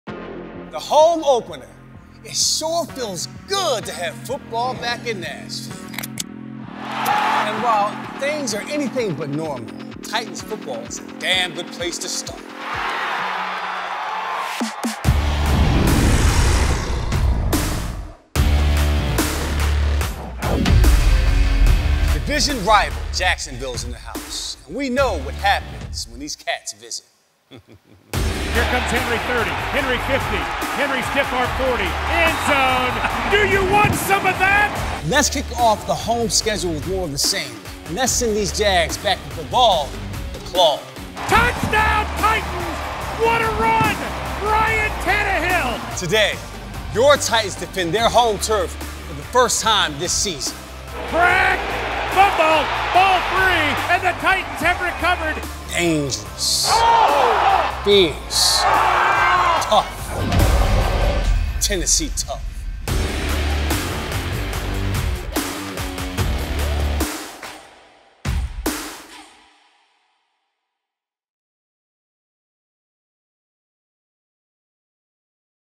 Eddie George: It's Time to 'Titan Up' Against the Jaguars
Former Titans running back Eddie George says it's time to 'Titan Up' against the Jacksonville Jaguars!